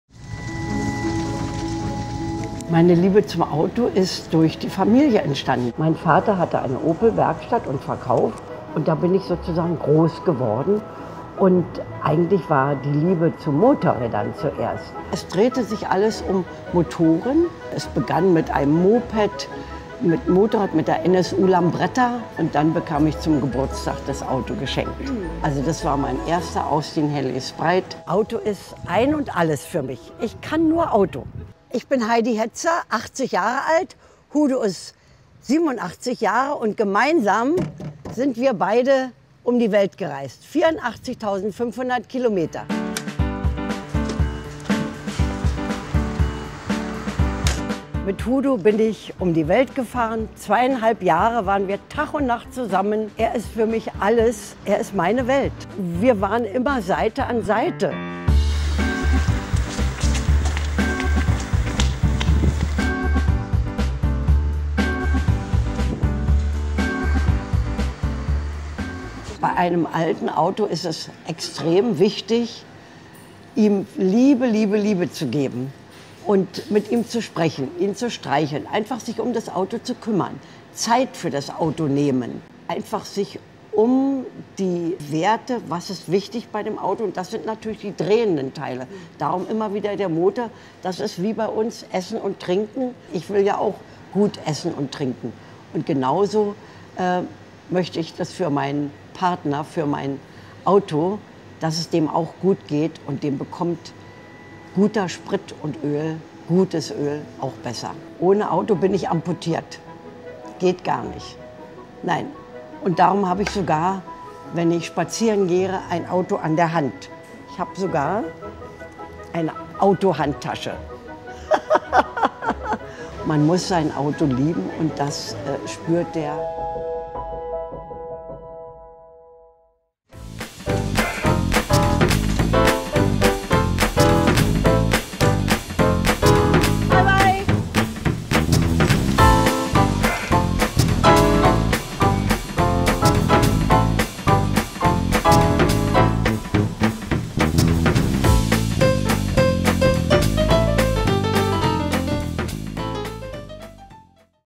Heidi Hetzer zu Besuch bei TOTAL
Heidi Hetzer hat uns mit ihrem Oldtimer "Hudo" besucht und erzählte von gemeinsamen Erlebnissen und ihrer Liebe zum Automobil.